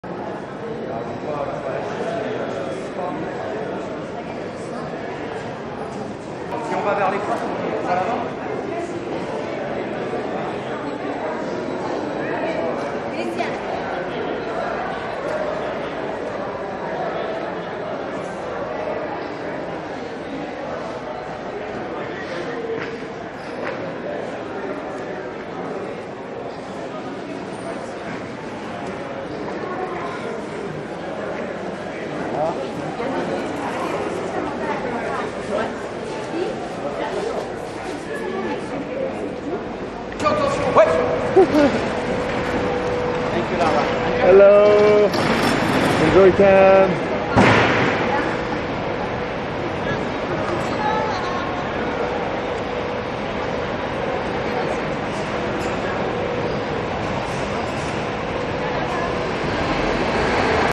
EXCLUSIVE : Lara Stone arriving at Cannes airport for the 2017 Cannes film festival
Lara Stone arriving at Cannes airport for the 2017 Cannes film festival

Cannes, France 23rd May 2017